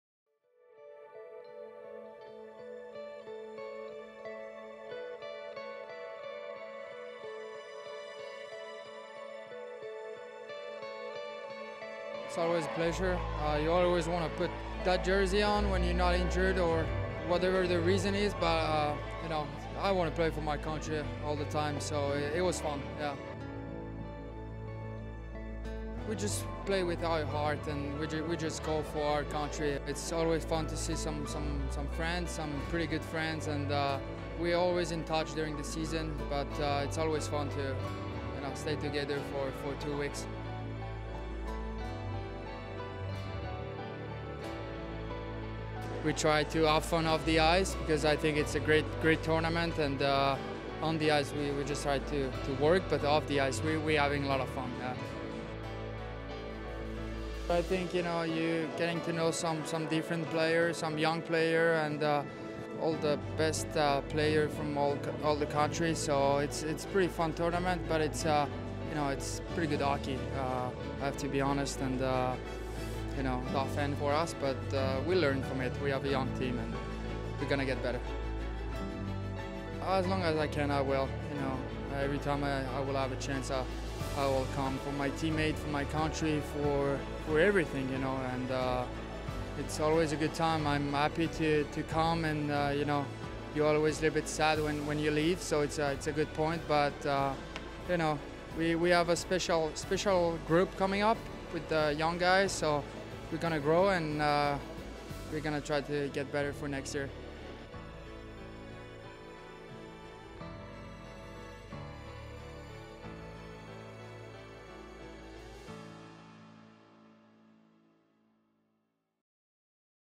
Beyond The Crest | Alexandre Texier (FRA) | 2023
For Alexandre Texier, representing France at the IIHF Ice Hockey World Championship is something he wants to do for as many years as he can.

The IIHF is proud to bring you the IIHF Ice Hockey World Championship!